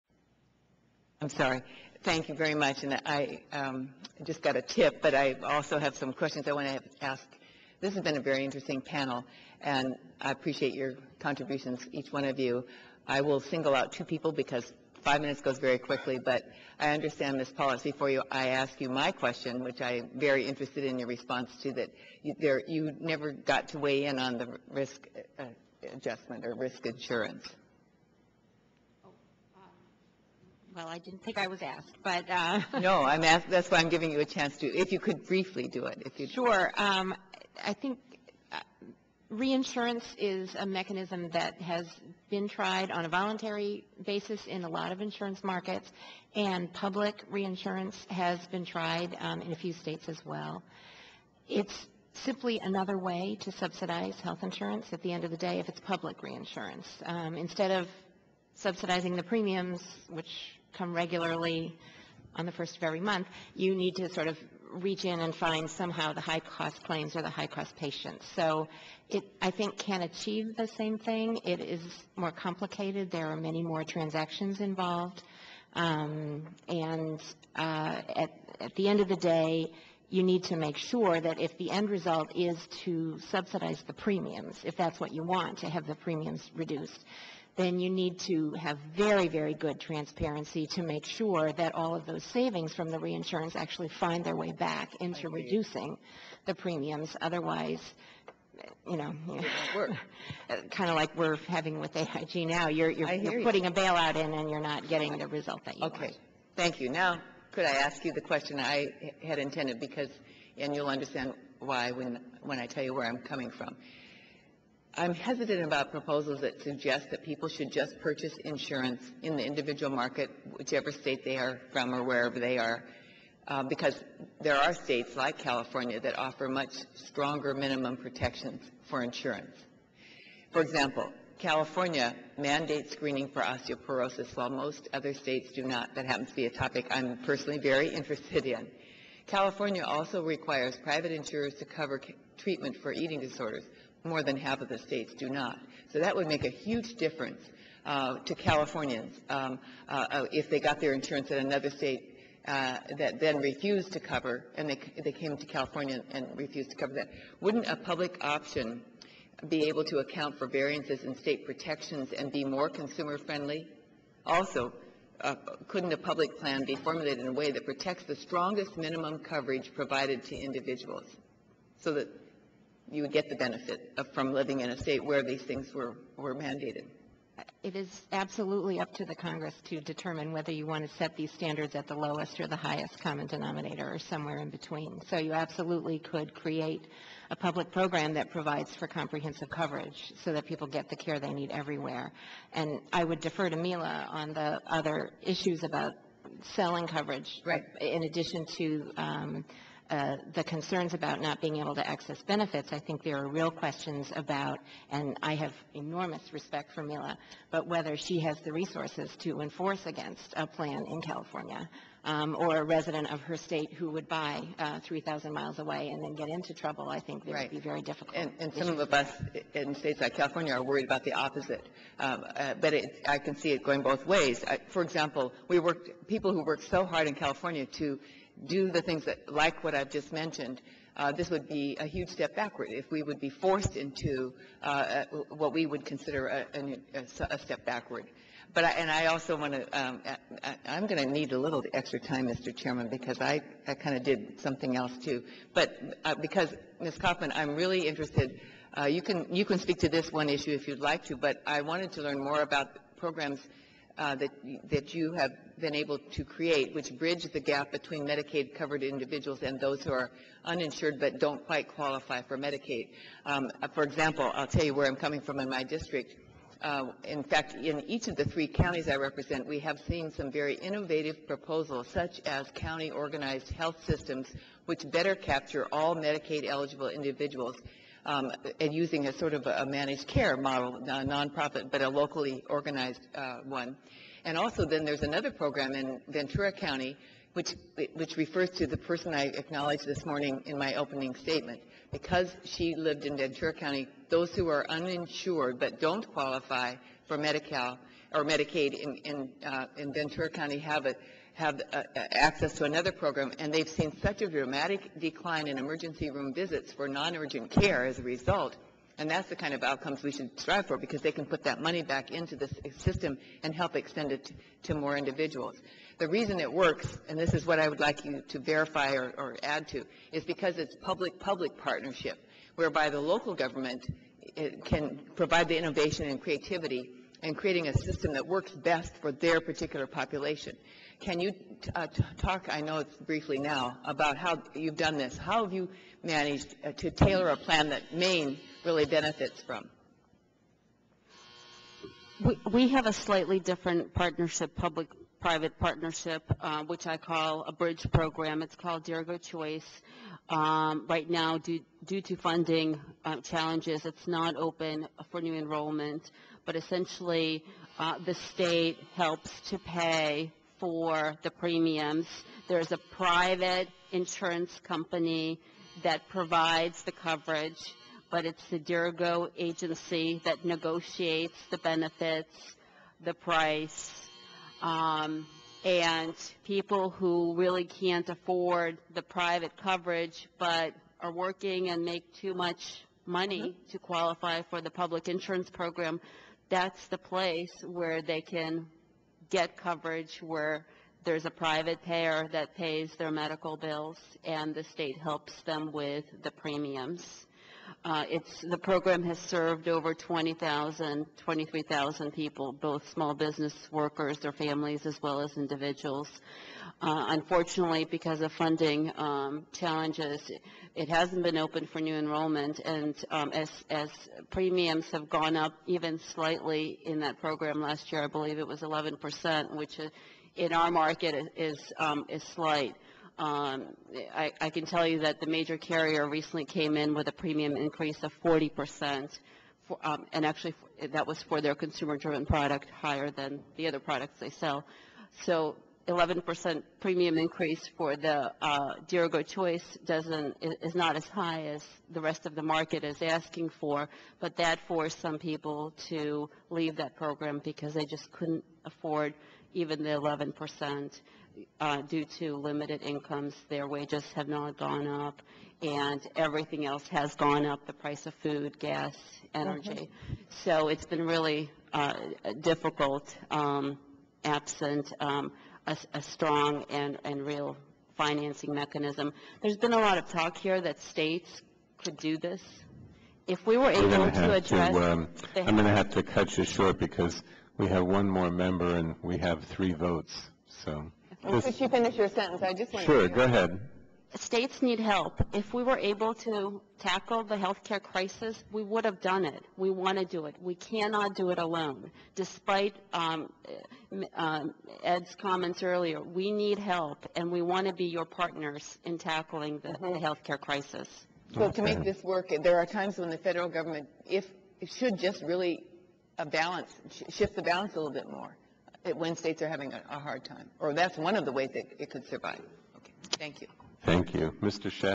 Ensuring Affordable Health Care - Questions2
Rep. Capps questions witnesses during an Energy and Commerce Committee hearing on ensuring affordable health care coverage (3/17/09)